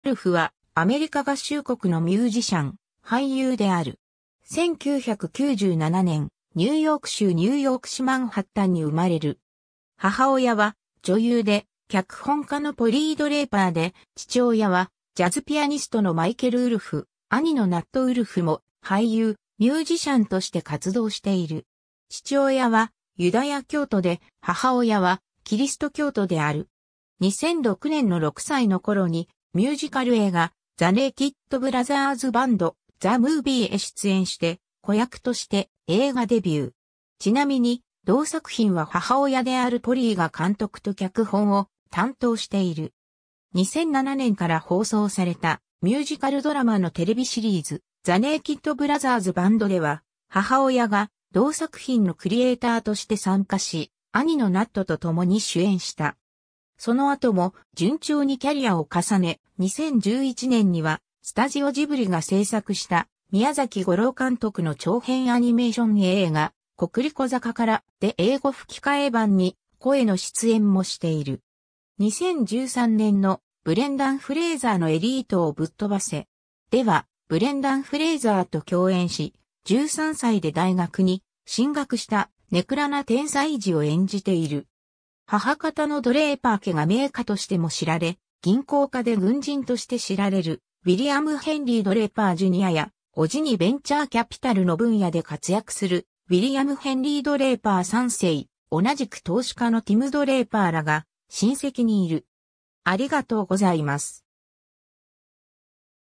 アレックス・ウルフ
アレックス・ウルフ（Alex Wolff, 1997年11月1日 - ）は、アメリカ合衆国のミュージシャン、俳優である。
 1997年、ニューヨーク州ニューヨーク市マンハッタンに生まれる。
母親は女優で脚本家のポリー・ドレイパーで、父親はジャズピアニストのマイケル・ウルフ、兄のナット・ウルフも俳優、ミュージシャンとして活動している。
父親はユダヤ教徒で母親はキリスト教徒である。
 2006年の6歳のころにミュージカル映画『The Naked Brothers Band: The Movie』へ出演して子役として映画デビュー。
ちなみに同作品は母親であるポリーが監督と脚本を担当している。
2007年から放送されたミュージカルドラマのテレビシリーズ『The Naked Brothers Band』では、母親が同作品のクリエイターとして参加し、兄のナットと共に主演した。
その後も順調にキャリアを重ね、2011年にはスタジオ・ジブリが製作した宮崎吾朗監督の長編アニメーション映画『コクリコ坂から』で英語吹替え版に声の出演もしている。
2013年の『ブレンダン・フレイザーのエリートをぶっとばせ!』では、ブレンダン・フレイザーと共演し、13歳で大学に進学した根暗な天才児を演じている。
 母方のドレイパー家が名家としても知られ、銀行家で軍人として知られるウィリアム・ヘンリー・ドレイパー・ジュニアや、叔父にベンチャー・キャピタルの分野で活躍するウィリアム・ヘンリー・ドレイパー3世、同じく投資家のティム・ドレイパーらが親戚にいる。